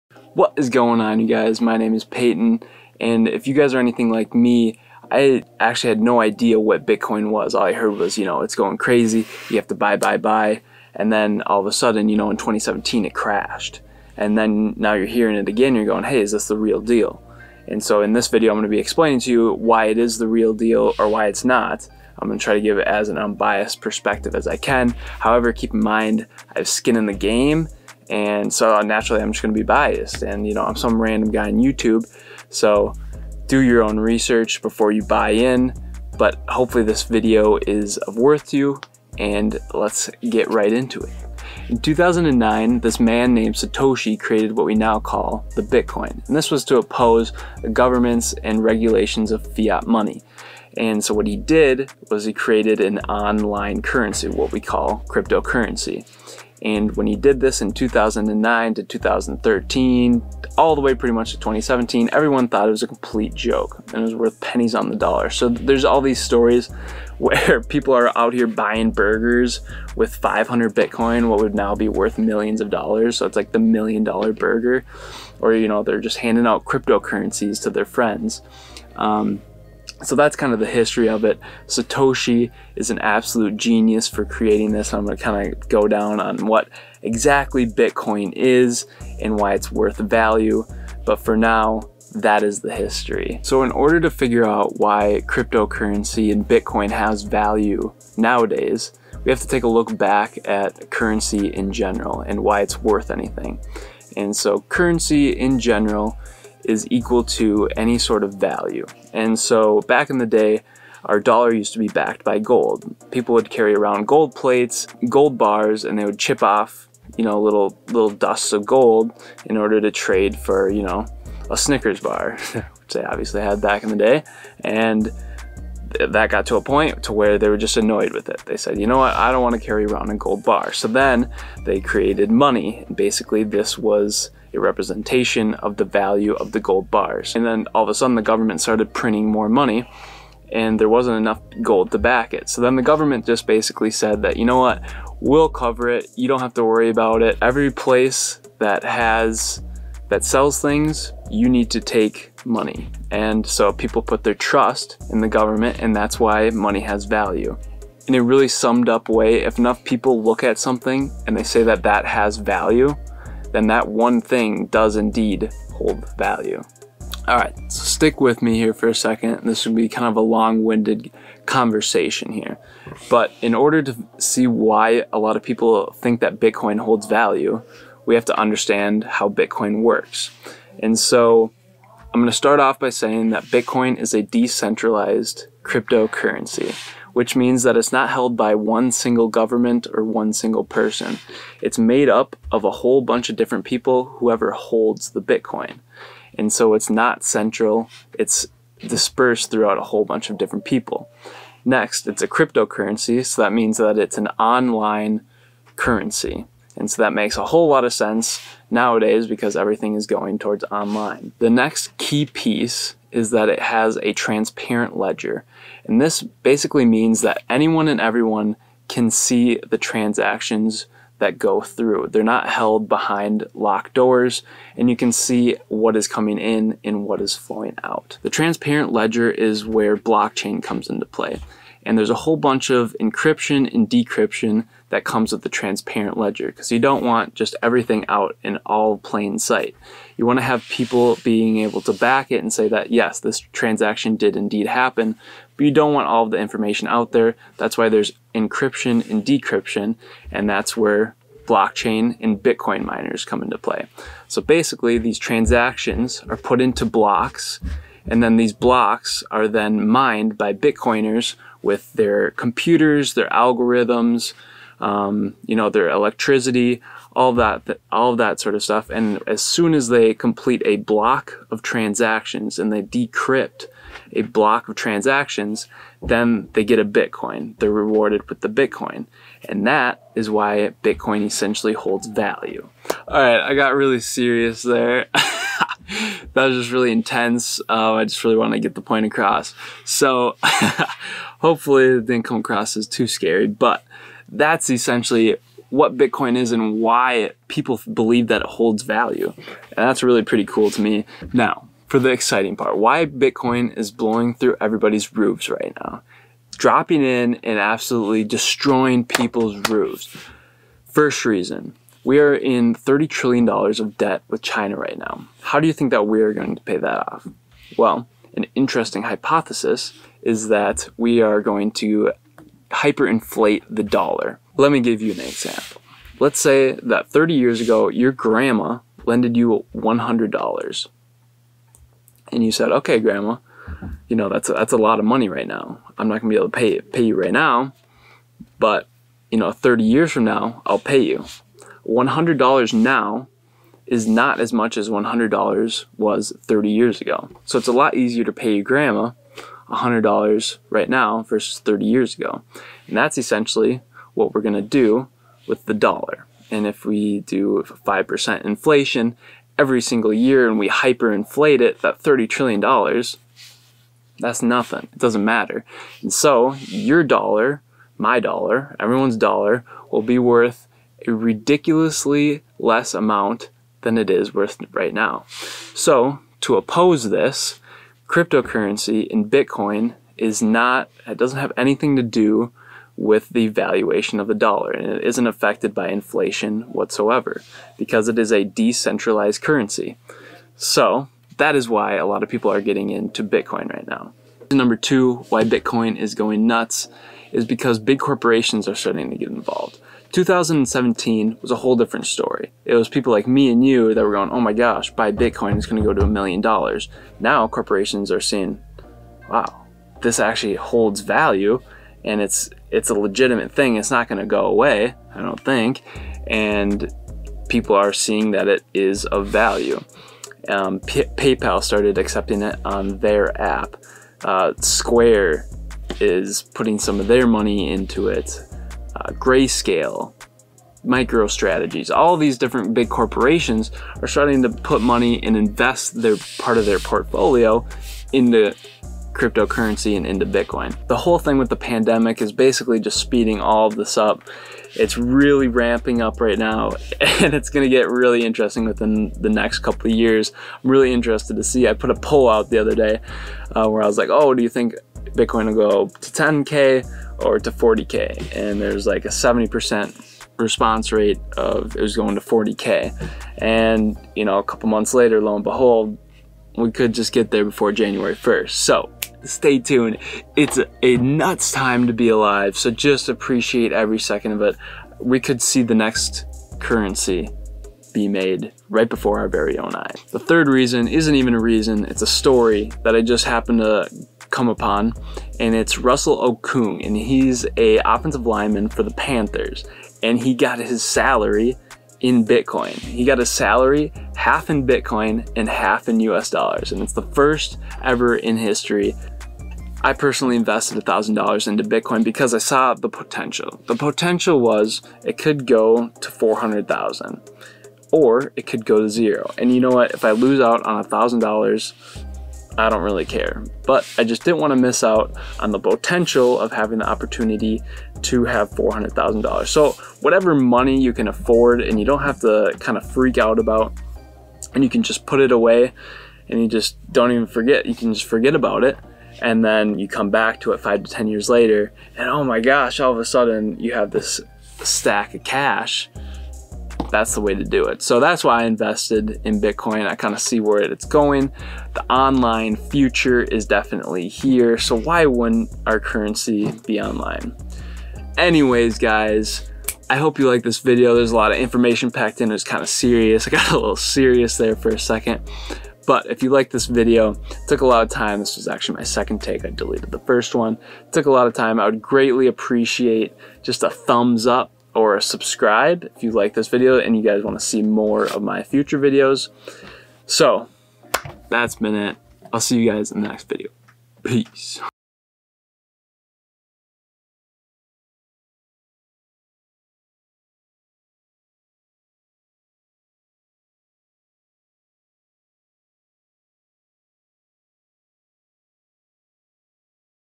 Bitcoin Explained (Why It Is Going Up Today!)
Bitcoin is hitting record highs and after what happened in 2017 you should understand why it is going up before you put hard earned money into it.